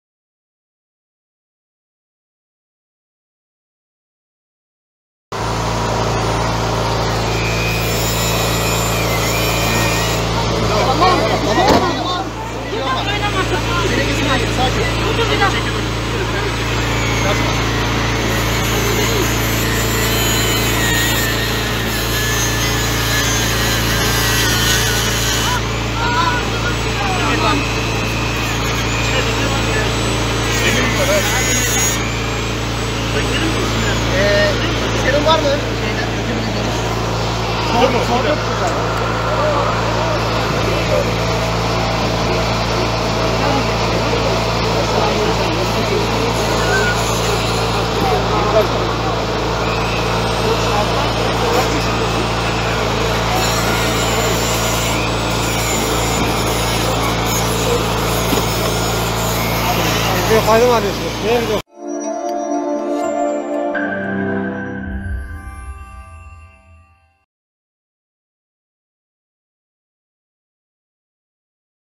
Bisikletin direksiyonu bacağına saplandı
Burhaniye Mahallesinde meydana gelen olayda devrilmesi sonucu bisikletin direksiyonu Kayra A.(13)'nın bacağına saplanması sonucu yaralandı. Yaralı çocuğu kurtarma operasyonu fahri muhabirimiz Erdi Çakır tarafından görüntülendi.